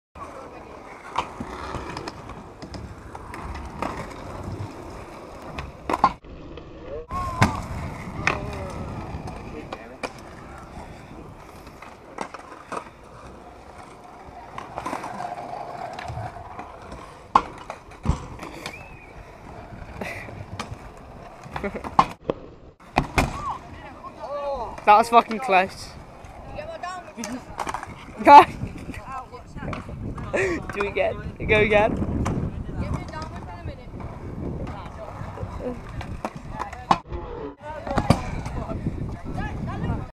Attempting butter cups GAY AS FUCK
READ
the quarter is really mellow and that kid "snaker" i.e Liam Westwood his a friend soo it was all a bit of a joke